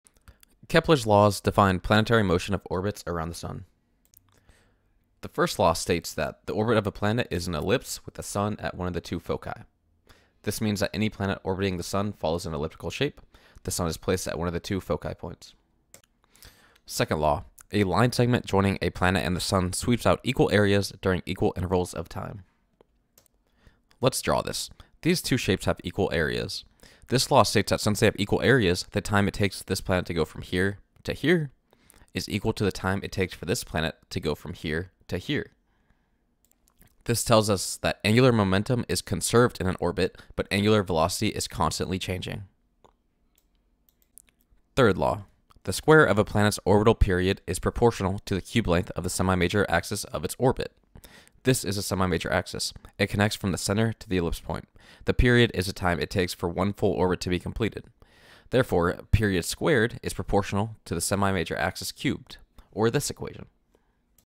Kepler’s Laws, Explained (Basics of Orbital Mechanics)
This video is about Kepler's three laws of planetary motion. This is essential to know for the basics of how orbit propagation occurs! This is the first of many future videos in orbital mechanics.

0:00 Kepler's First Law
0:16 Kepler's Second Law
0:44 Kepler's Third Law